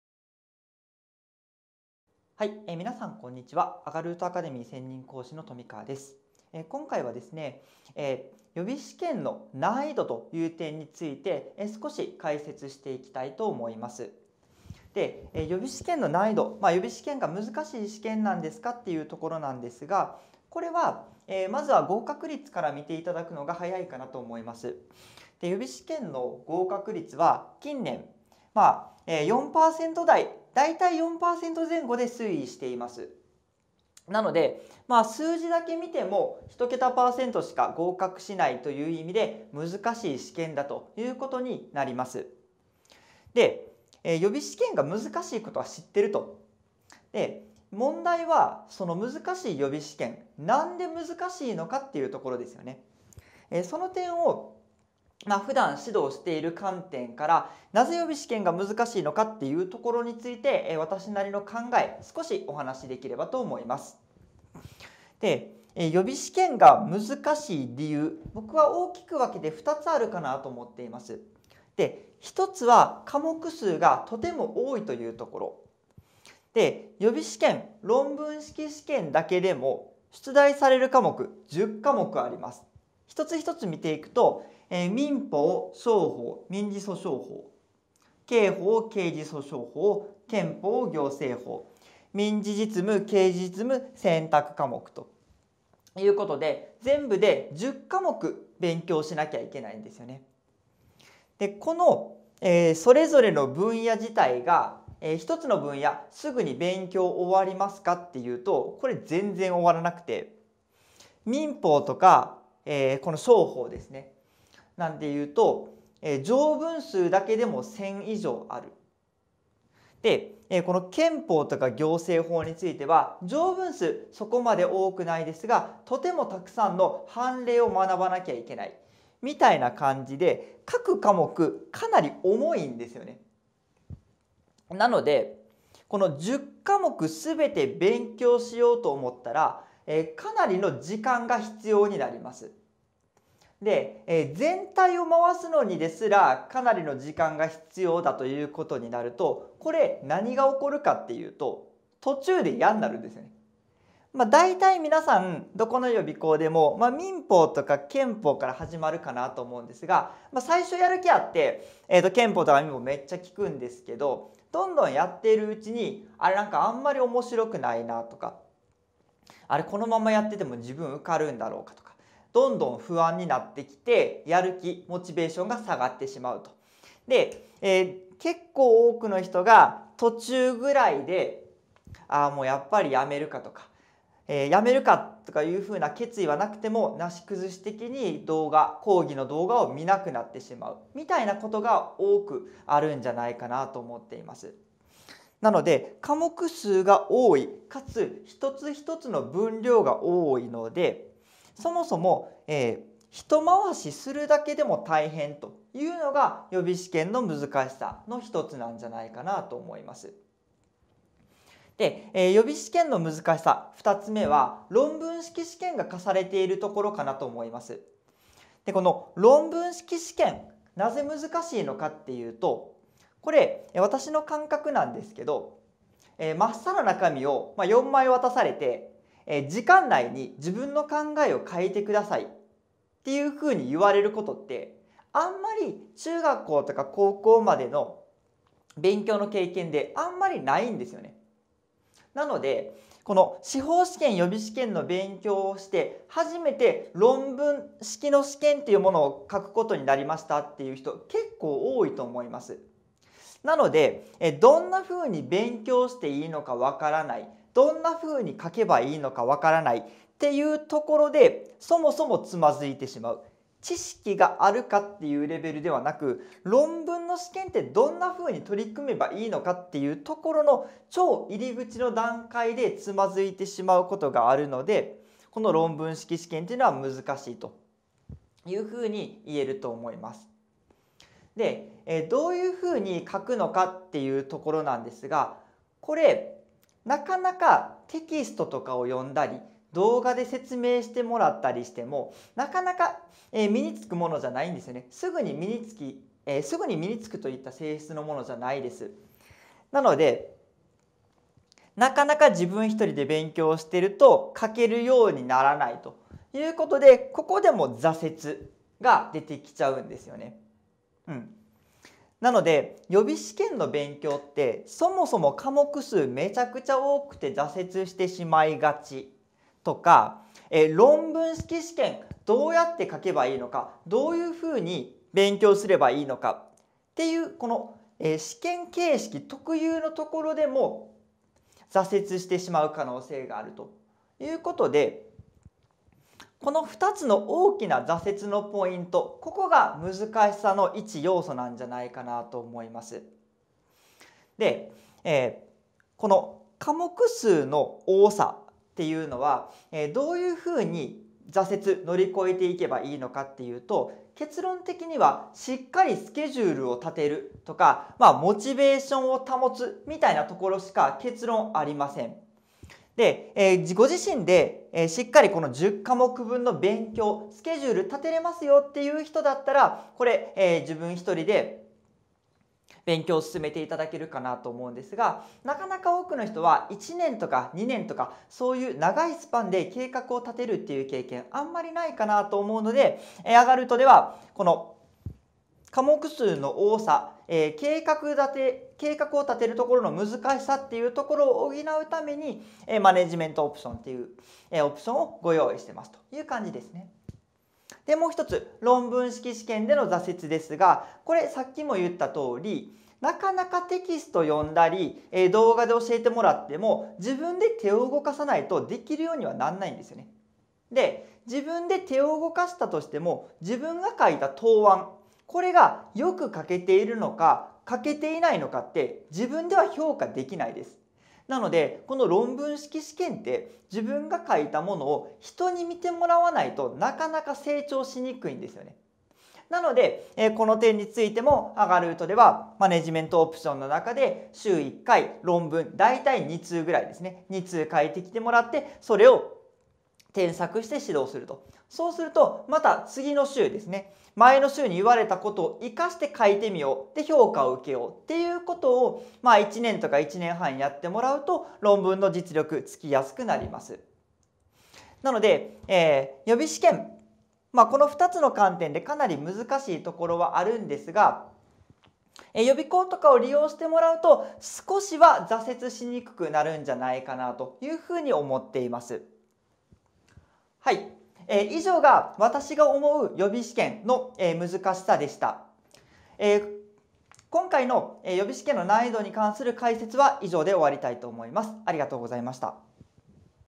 【富川純樹講師が解説！】司法試験予備試験の難易度はなぜ高い？「途中で嫌になる」理由と挫折しない方法
▼こちらのコラムでより詳しく説明しています。

アガルートアカデミー司法試験の富川純樹講師が、司法試験予備試験の難易度について解説します。
講師からみた難易度が高い理由、そして予備試験に合格するためのポイントを紹介していますので、ぜひご覧ください。

■アガルートアカデミーSNS情報
▽Twitterアカウント一覧